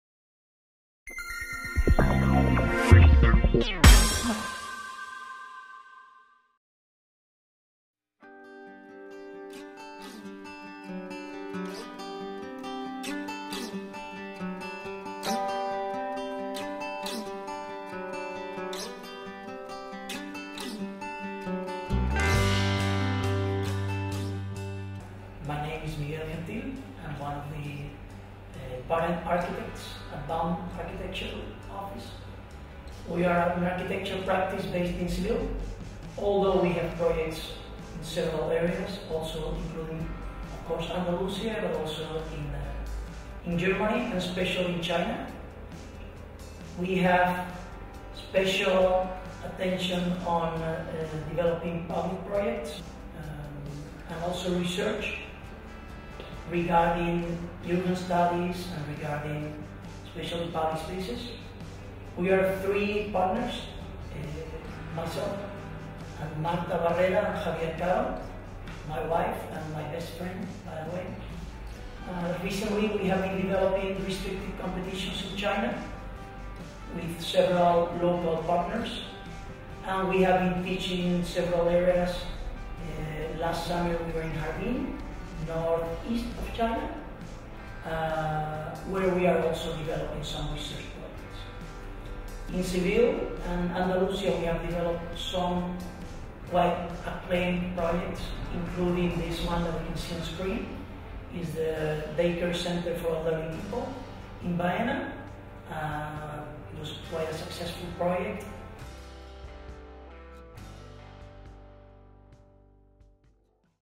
Baum arquitectura, un estilo muy internacional
Marta Barrera, Miguel Gentil y Javier Caro son los fundadores de Baum, un estudio de arquitectura innovador ubicado en Sevilla. Desde su sede en en La Alfalfa desarrollan proyectos nacionales e internacionales para países como Alemania o China. Es en este último donde han logrado desde hace diez años un gran desarrollo profesional de dimensión internacional con sus grandes proyectos arquitectónicos para instituciones y empresas chinas. El más importante es la ‘Ciudad del Conocimiento’ en Guiyang, capital de la provincia de Guizhou: un distrito universitario para 50.000 estudiantes, actualmente ya en construcción, en 240 hectáreas. A este se suman importantes proyectos en Shanghai o Hangzhou.

De forma paralela, los tres fundadores de Baum crearon La Casa China, un espacio cultural dedicado a la cultura de dicho país donde los ciudadanos pueden aprender el idioma o conocer más a fondo la costumbres chinas a través de exposiciones, conferencias, cine, cuentacuentos,…